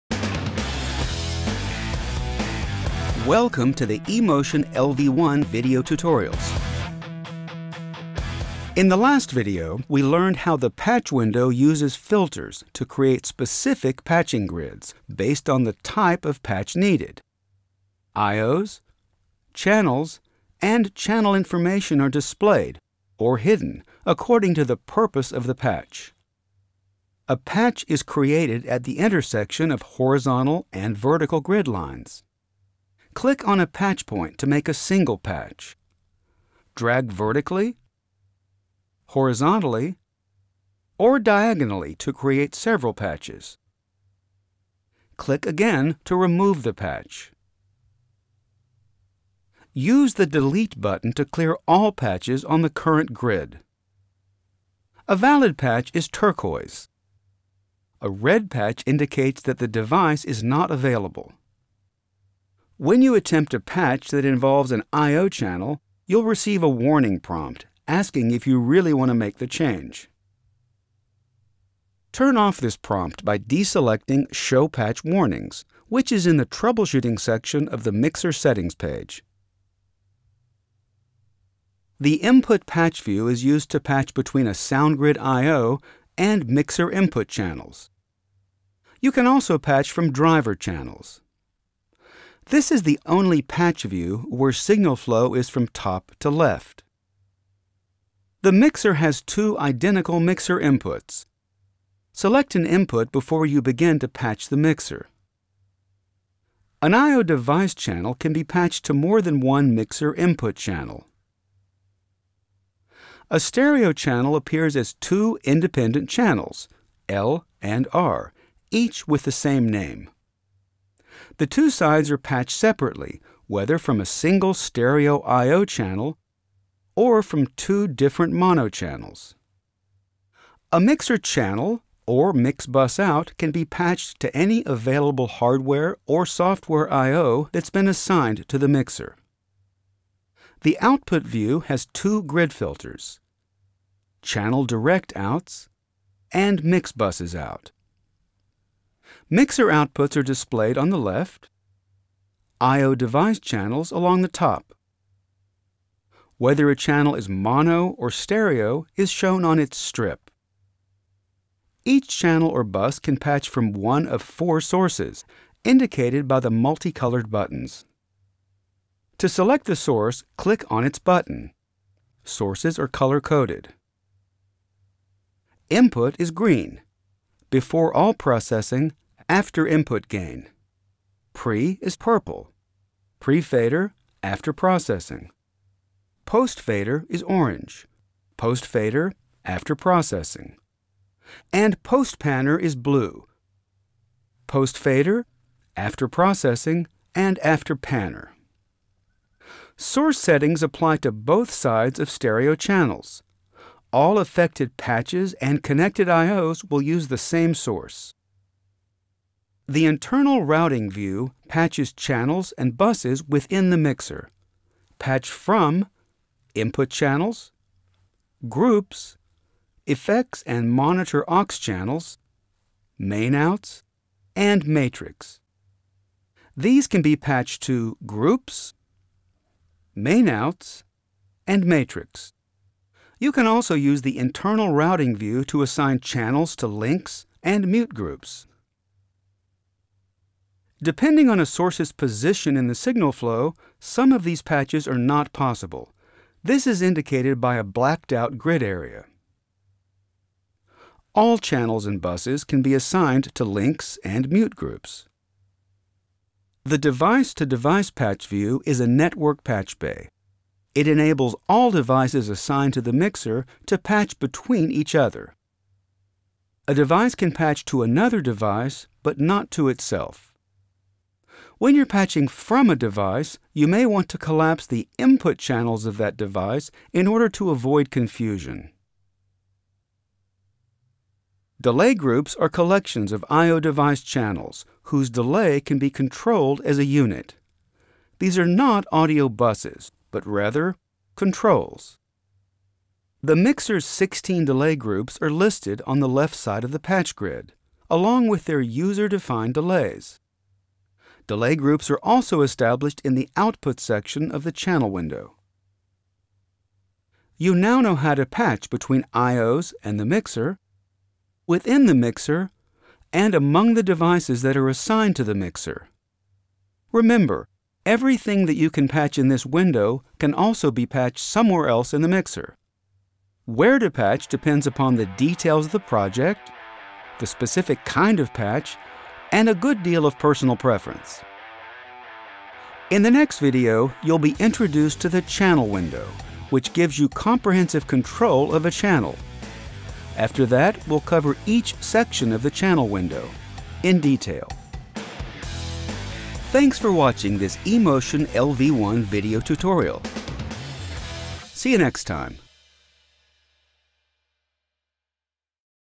eMotion LV1 Tutorial 2.2: Using the Patch Window
Learn how to route patch configurations between I/Os, channels, auxes, groups, matrixes and effects in your eMotion LV1 live mixer system.